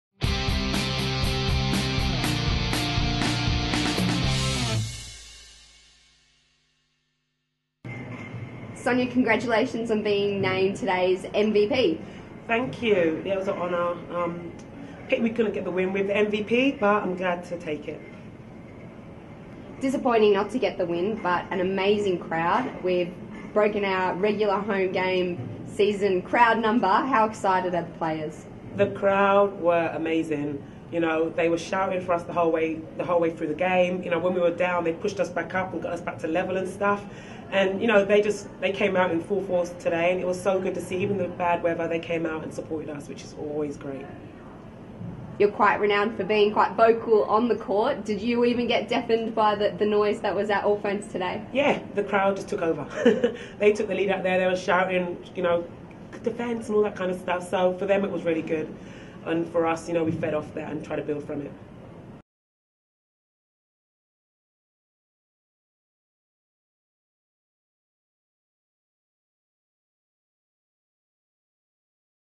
NSWSwiftsTV: Round 11 Reaction - Sonia Mkoloma
NSWSwiftsTV spoke to Swifts defender Sonia Mkoloma after she was named MVP in the Swifts v Thunderbirds blockbuster at Allphones Arena.